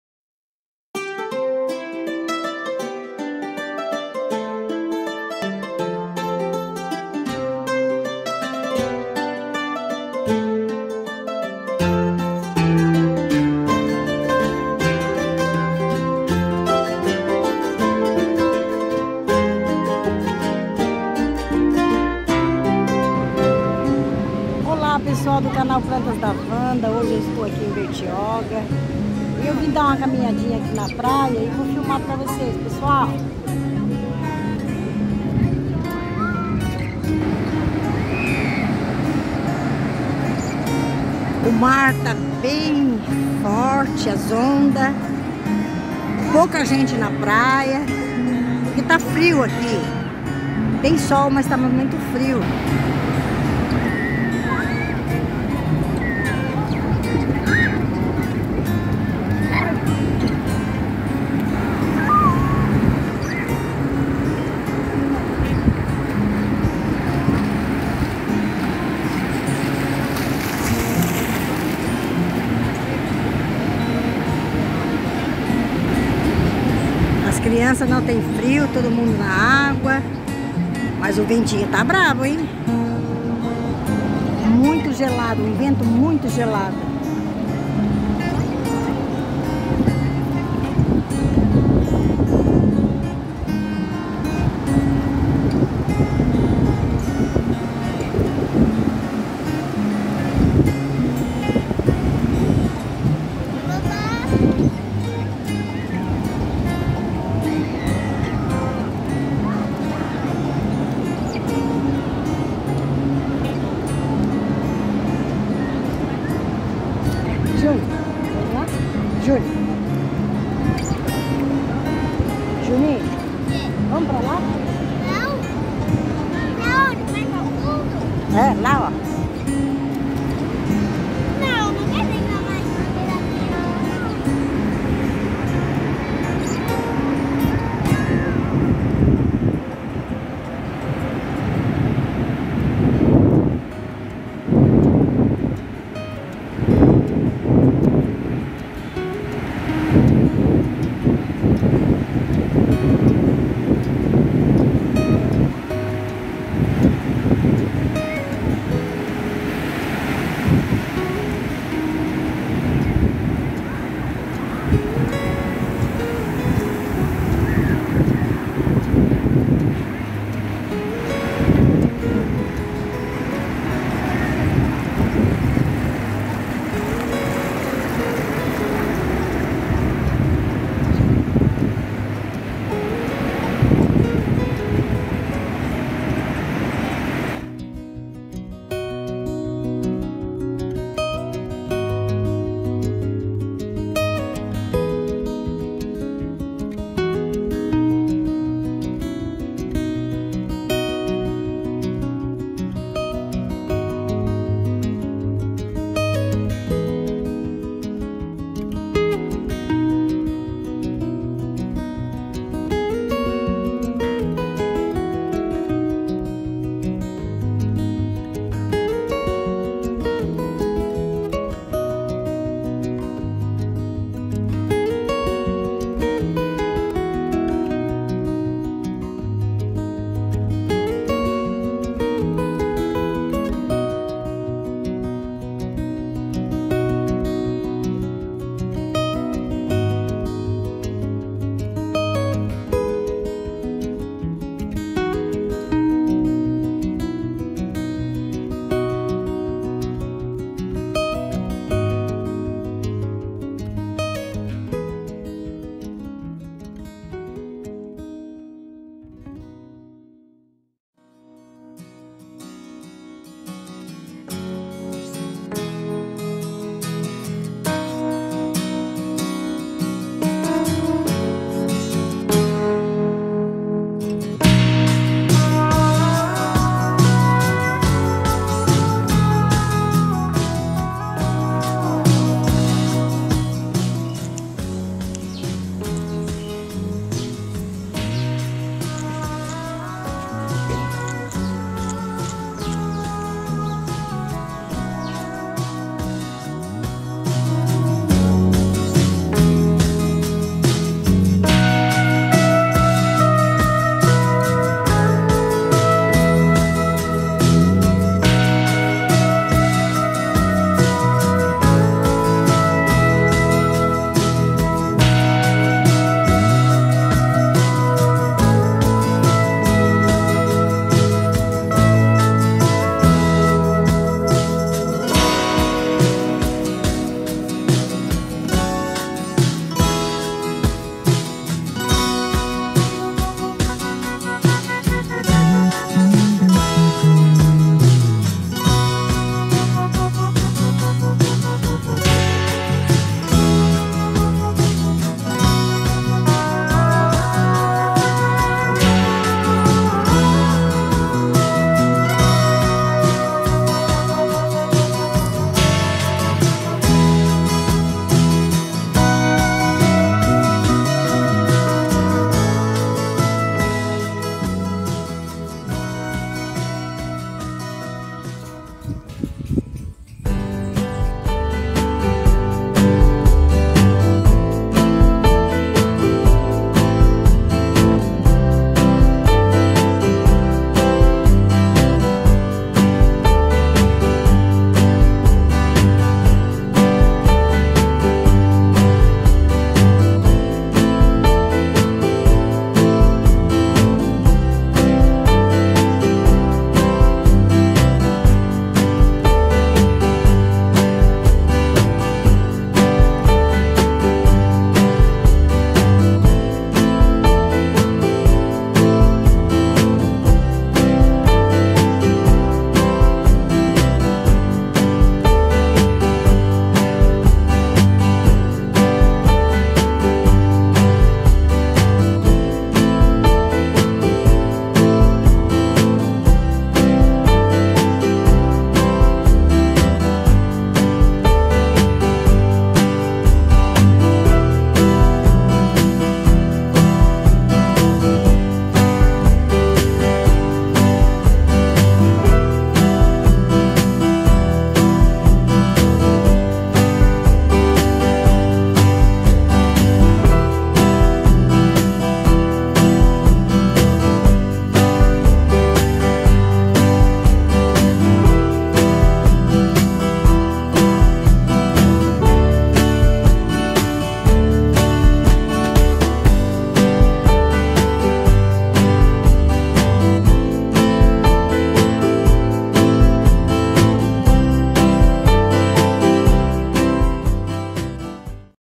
CAMINHANDO NA PRAIA
CAMINHANDO PELA PRAIA E PELO BAIRRO DE VISTA LINDA CONTEMPLEI LINDAS FLORES